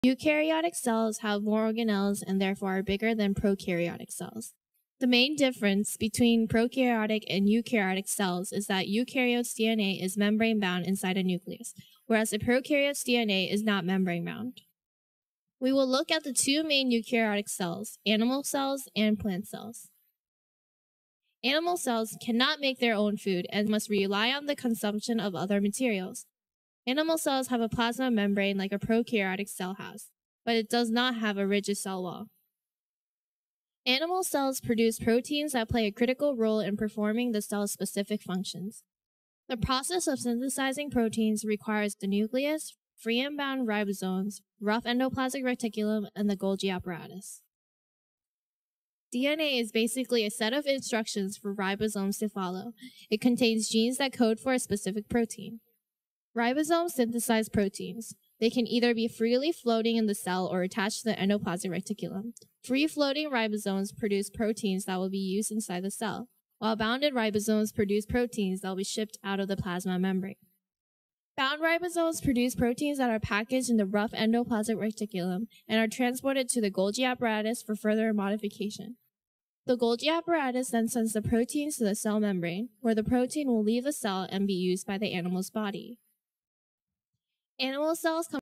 Eukaryotic Animal Cell Part 1
The file size of this video was a bit too large so I had to split it into two parts.

Cells and Their Organelles is one of two 3D animations created by James (JB) Wills and narrated by Sarah Lim, two high school juniors. The videos were created for a Capstone Project at Clarksburg High School using several different professional software packages and comprised over 200 hours of work. The project started with a story board for the production and each cell had to be drawn and animated and narration had to be overlaid in the final production. It is being used for instructional purposes by AP Biology classes at Clarksburg High School.